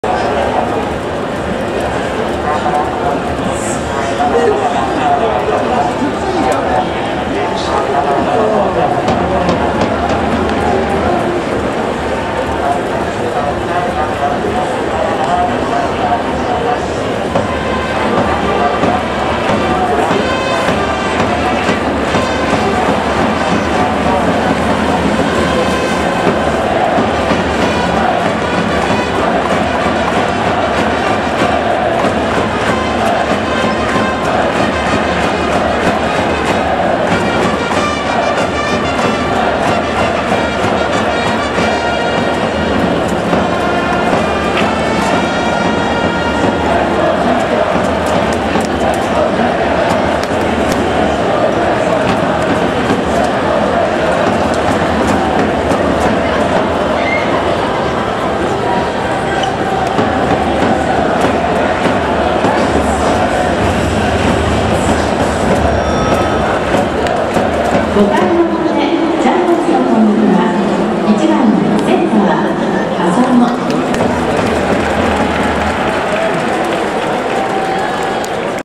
福田と堂上のプラカード掲げる山本拓実＆郡司裕也 グラウンドへ手を振る京田陽太 2023／10／3
2023／10／3 中日vs巨人　1−3 引退セレモニー